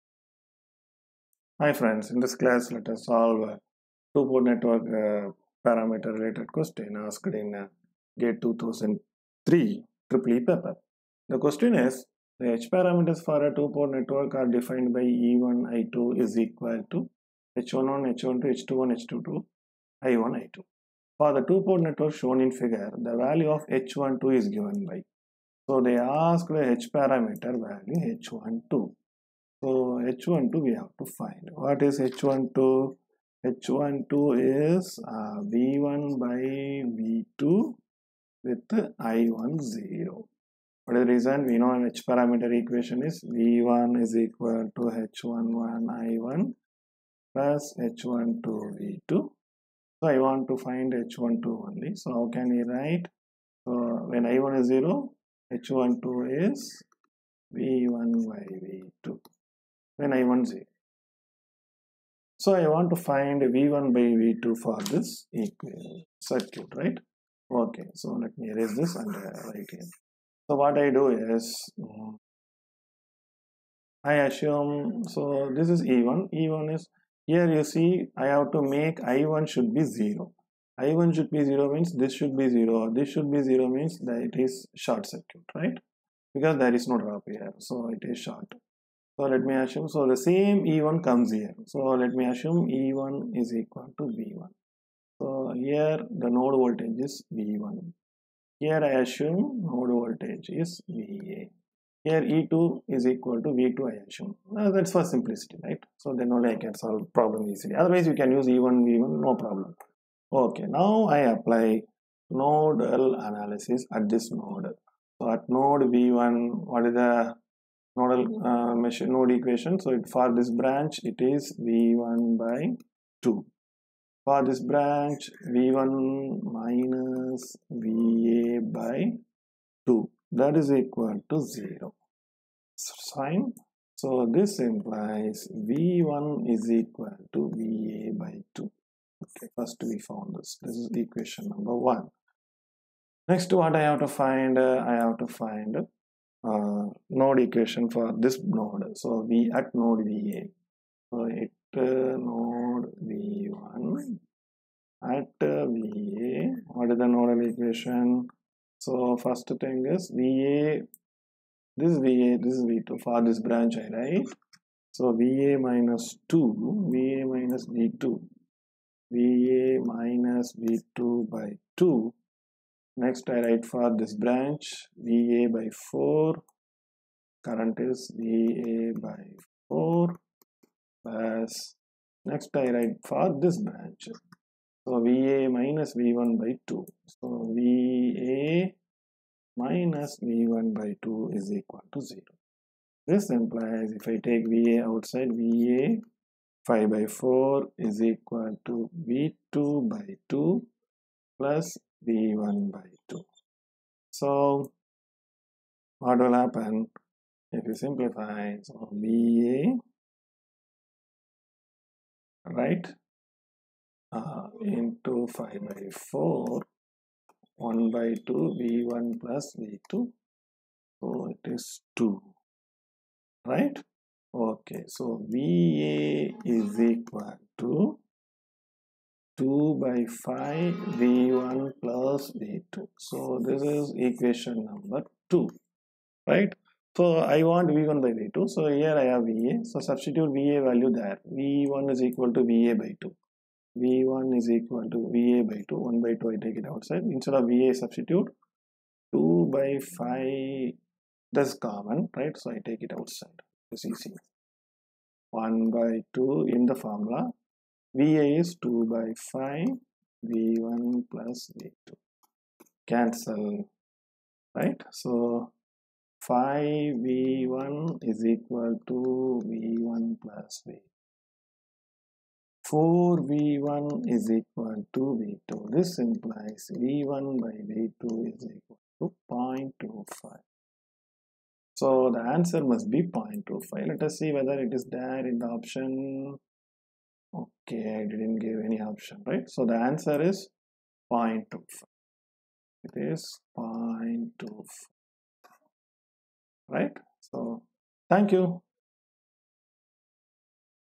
Network Theory GATE 2003 EE Two Port Networks |#84
Network theorems related GATE Exam problem is solved in this video @Majeeth Lectures . Here GATE2003EE question is solved

@Majeeth Lectures